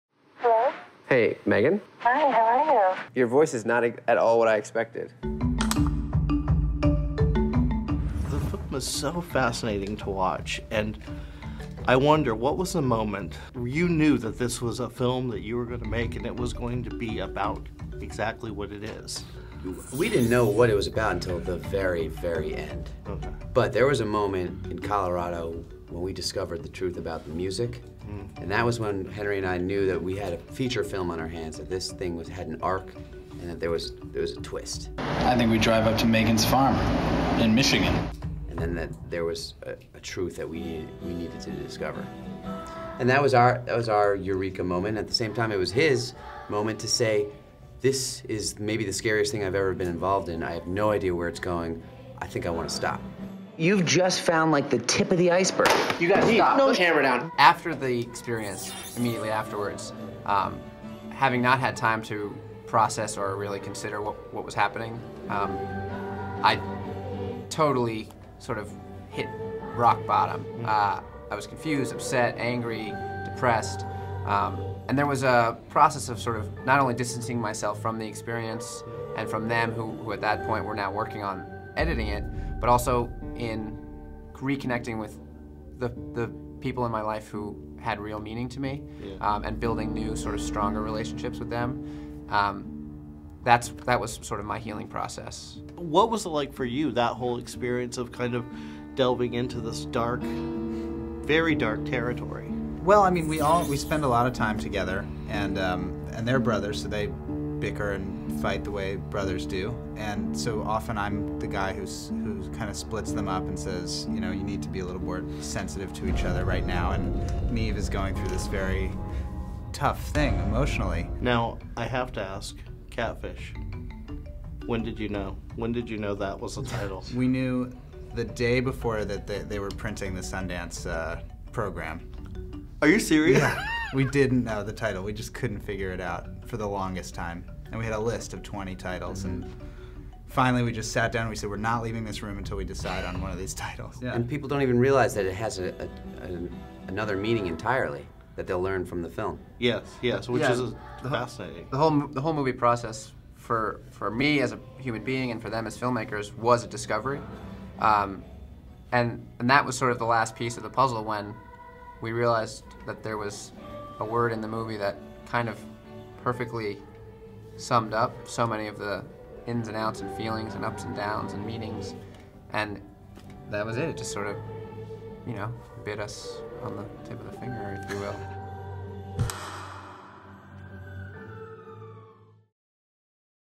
Catfish Rogue Movie Interview
iamROGUE's JimmyO interviews the creators of the film, CATFISH about their film opening in theaters SEPTEMBER 17th, 2010!!